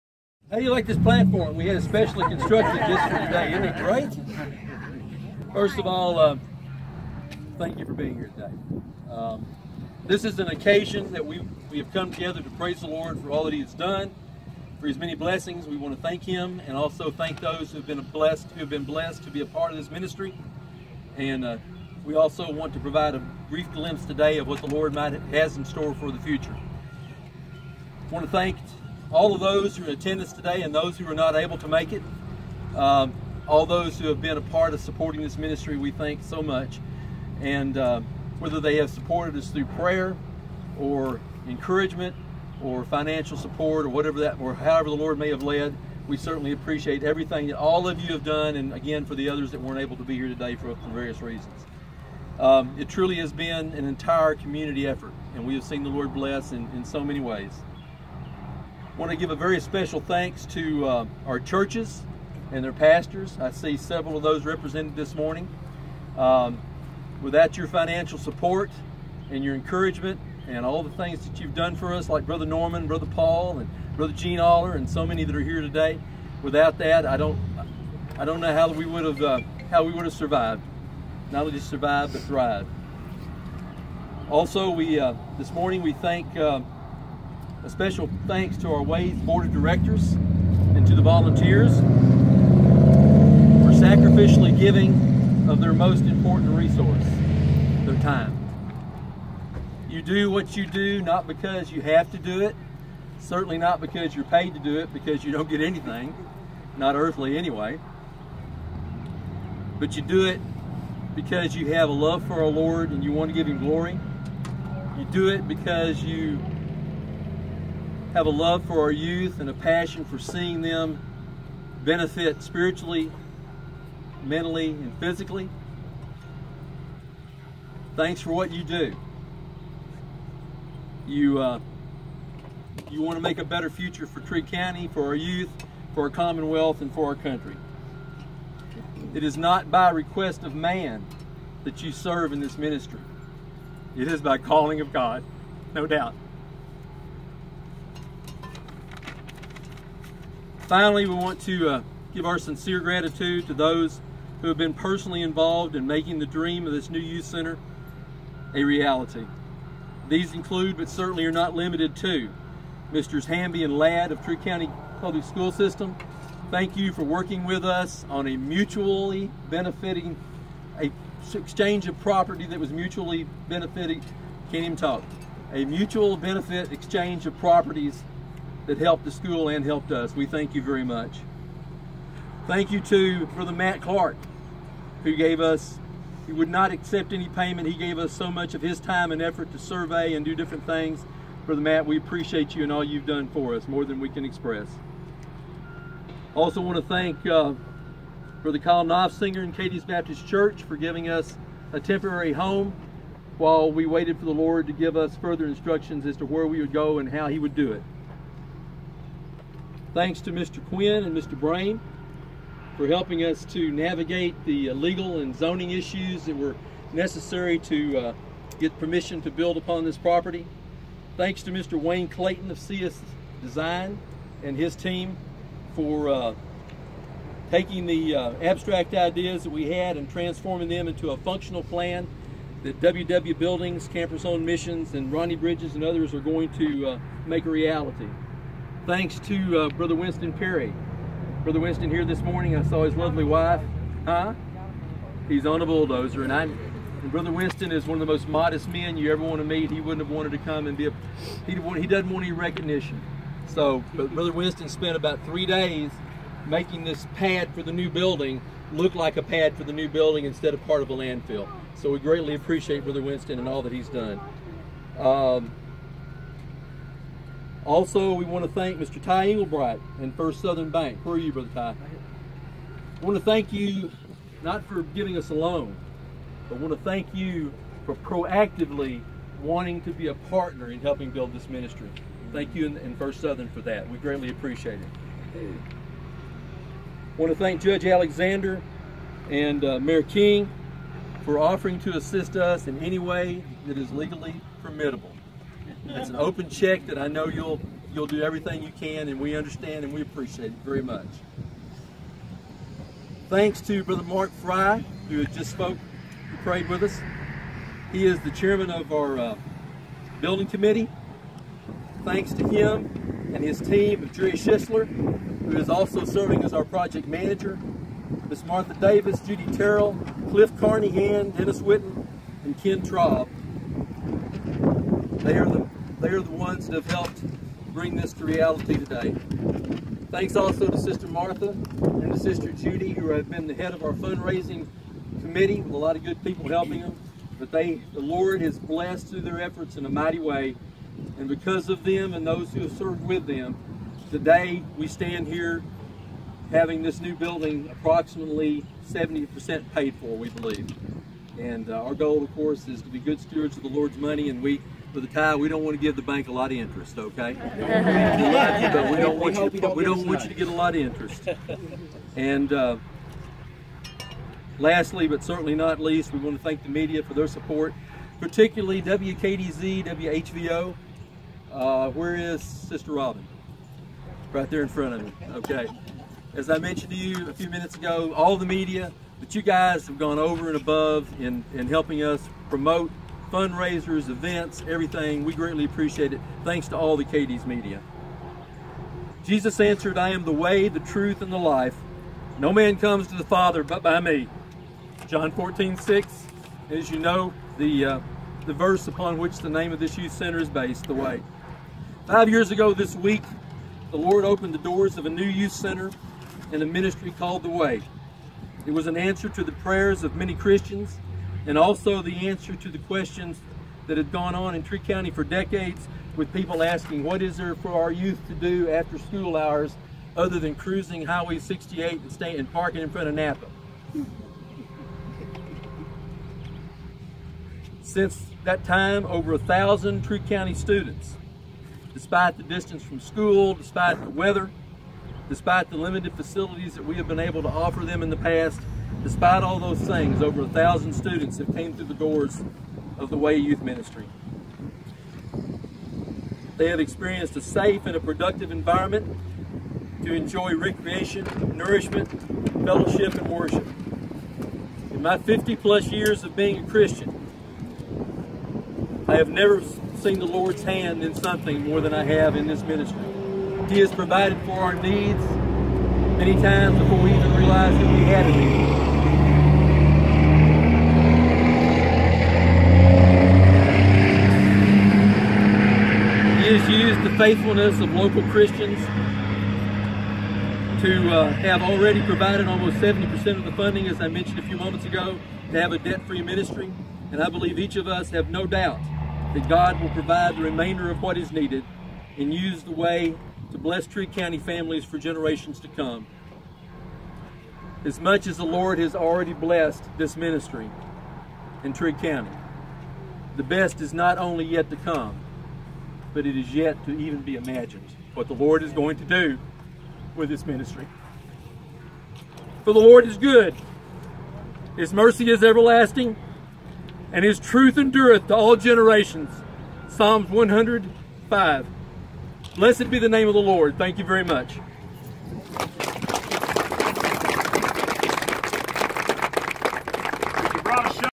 The Way breaks ground
The Way Christian youth ministry broke ground Friday on a new 11,800 square-foot youth center at 197 Lafayette St. in Cadiz. Volunteers and members of the community attended the event, which included a brief reception thereafter.